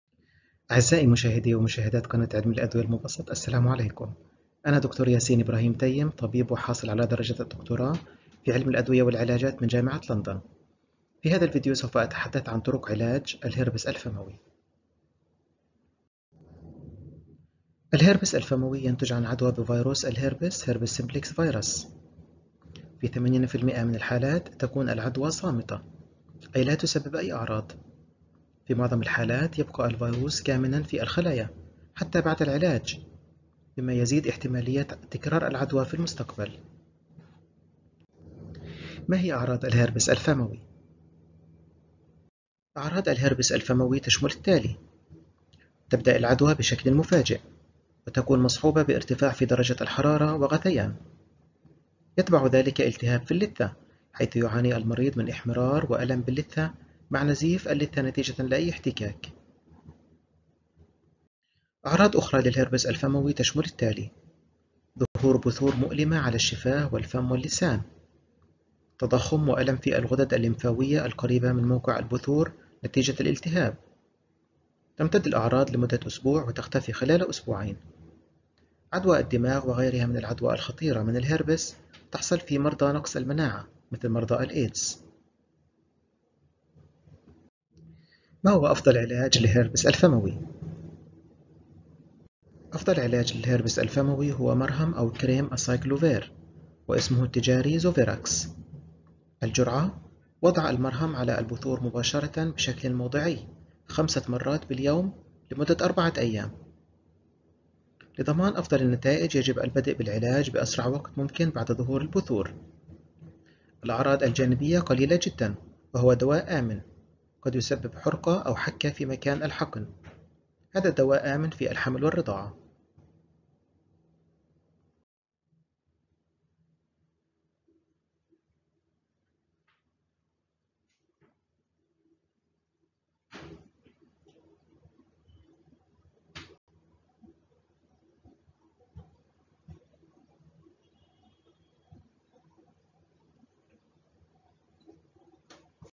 كيف أعالج الهربس الفموي؟ د. ياسين ابراهيم تيم
العديد من الناس يعانون من عدوى الهربس الفموي وظهور بثور مؤلمة جدا على الفم والشفاه. في هذا الفيديو سوف نتعرف على أفضل علاج للهربس الفموي